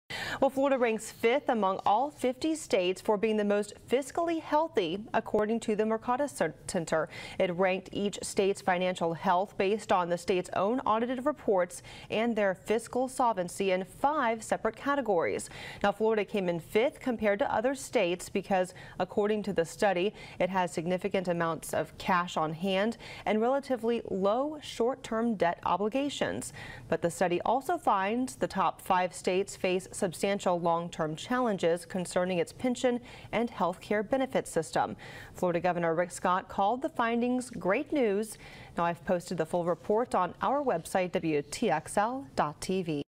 Florida Ranks High in Fiscal Health
Florida ranks 5th among all 50 states for being the most fiscally healthy, according to the Mercatus Center.